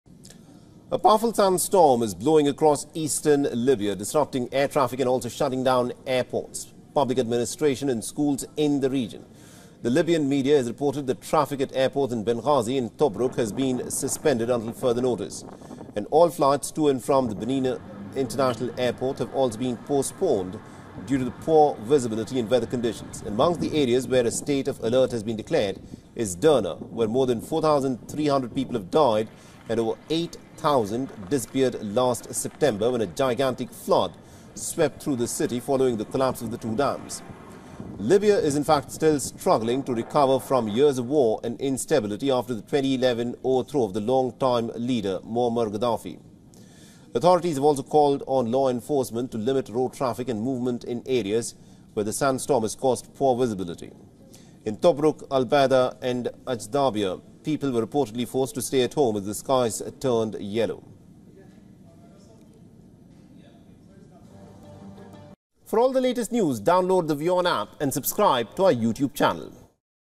Sandstorm hits East Libya | Climate Tracker | Latest English News | WION
A powerful sandstorm is blowing across eastern Libya, disrupting air traffic and shutting down airports, public administration, and schools in the region...